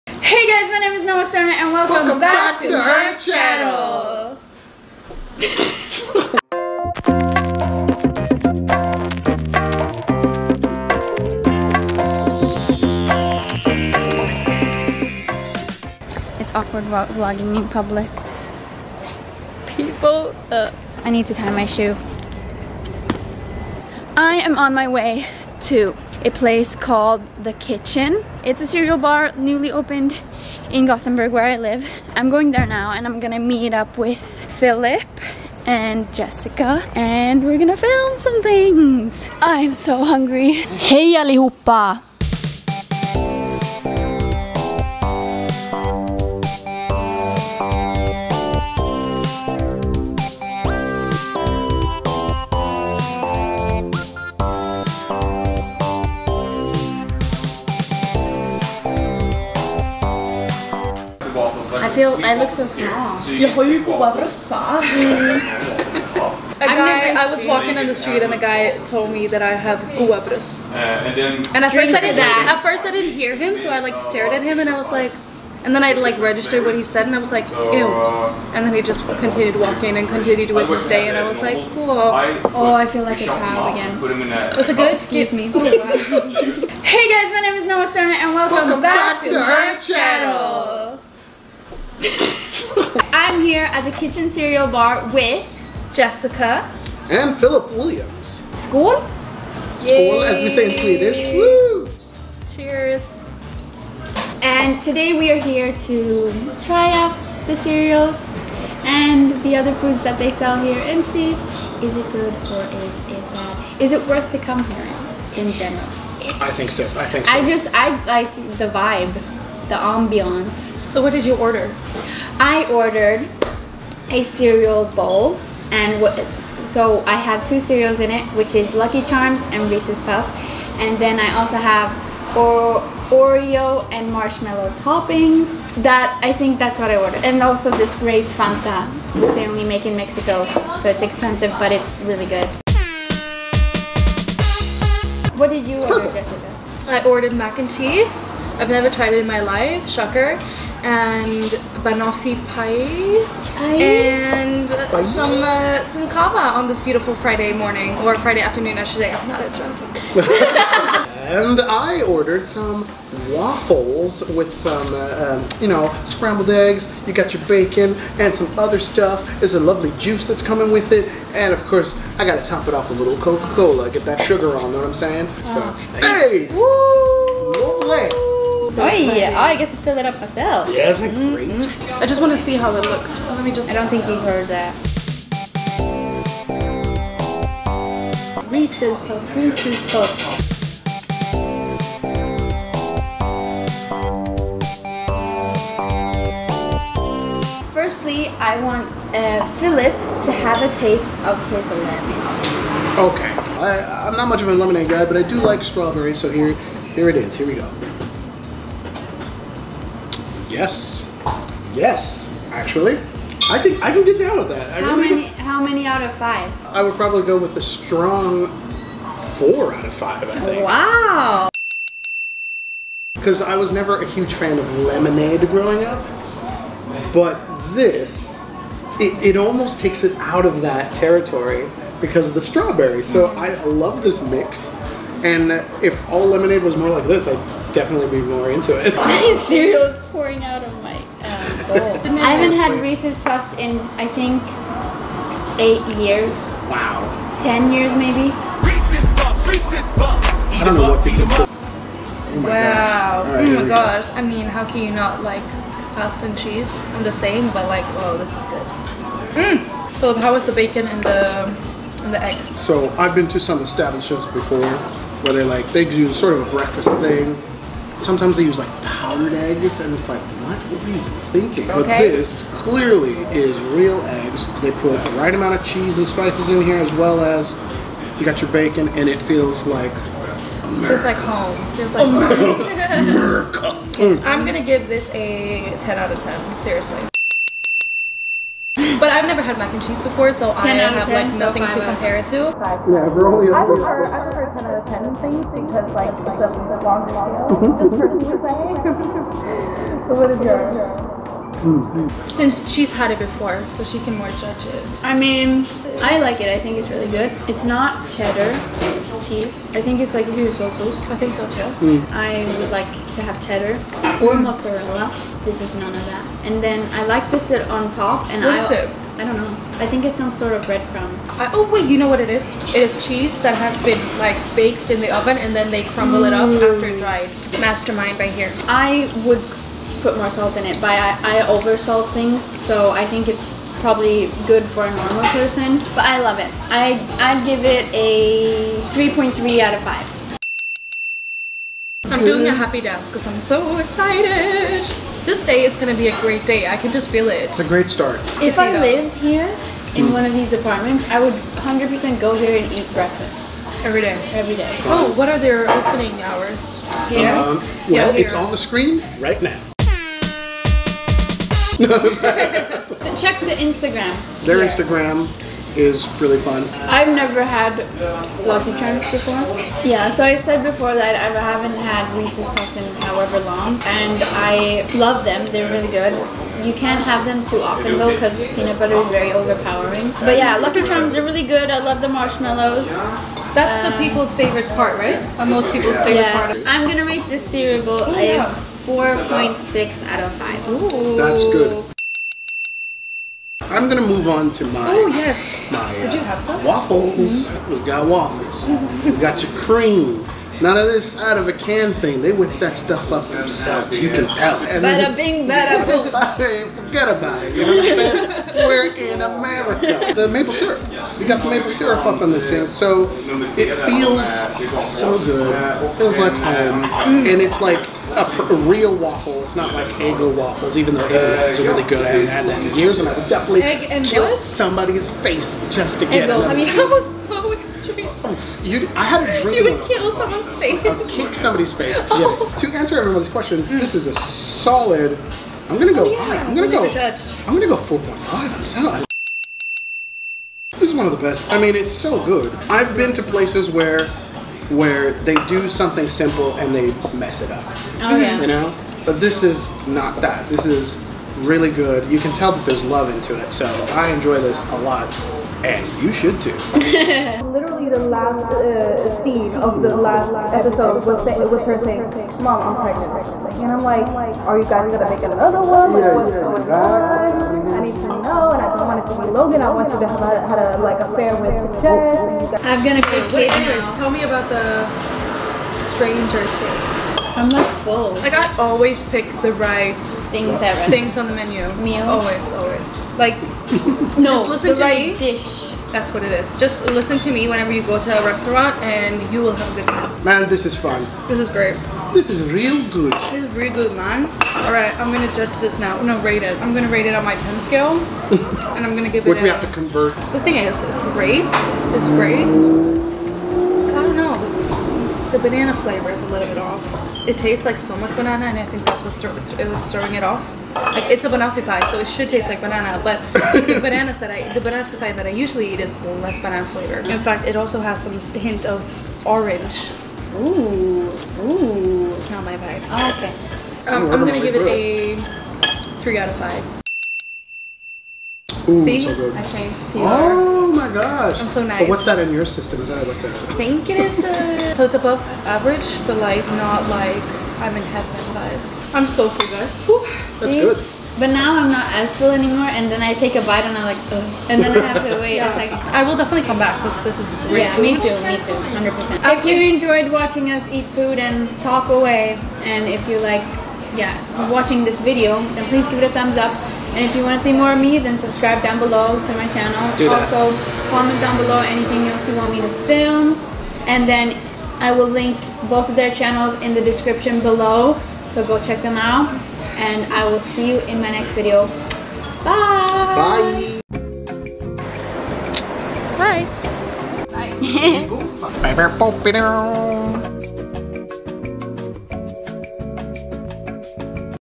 TheKitchen MUKBANG w/ FILLIP & JESSICA!
hey guys! in this week's video, you get to watch me, Fillip and Jessica eat and rate our food at the amazing new cereal bar TheKitchen in Gothenburg!

Socials (IG/Twitter/Snapchat/Tumblr/TikTok): @noasterner